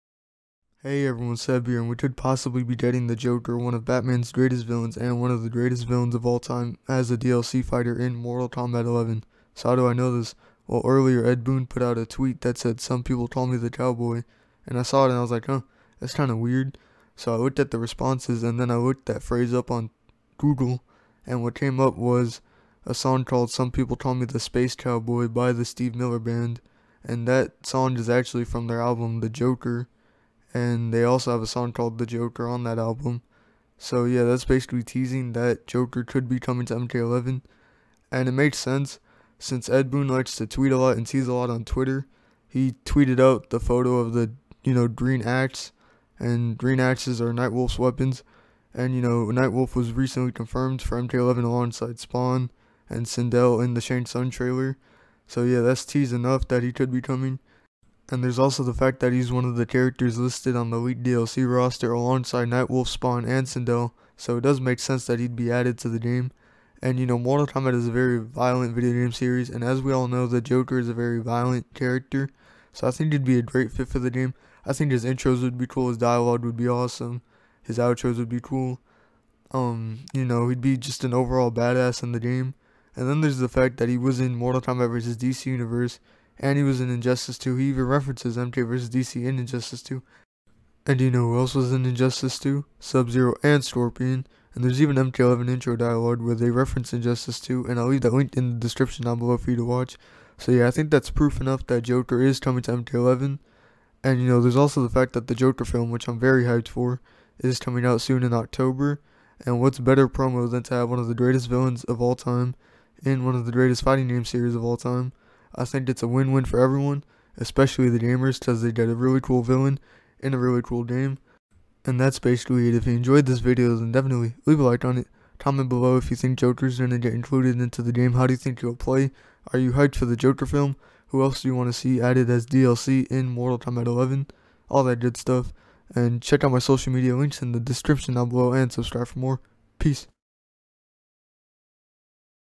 Mortal Kombat 11 - Joker DLC Character TEASED?
This video is about a possible Joker DLC character tease for Mortal Kombat 11. I talk about a tweet that Ed Boon released teasing Joker in MK11.